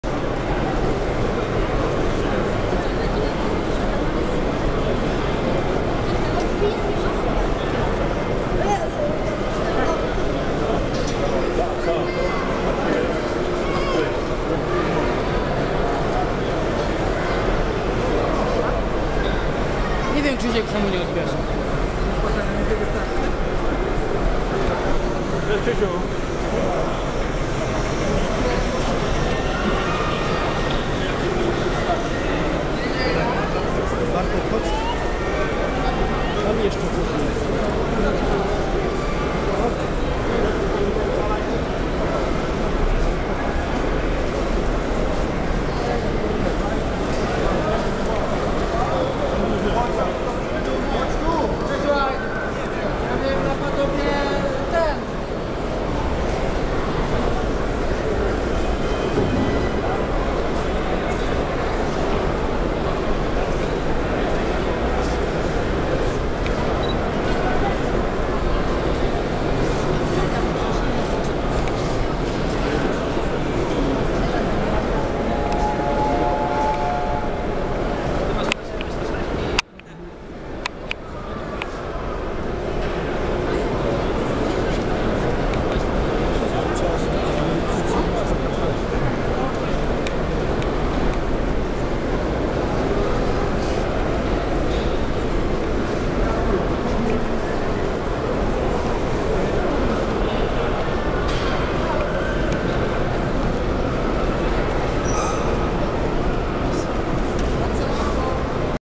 Makieta Modułowa o wielkości 2 tys. m2 - HOBBY 2010
Tylko podczas HOBBY 102 modelarzy z całej Polski zaprezentuje Makietę Modułową o powierzchni blisko 2 tys. m2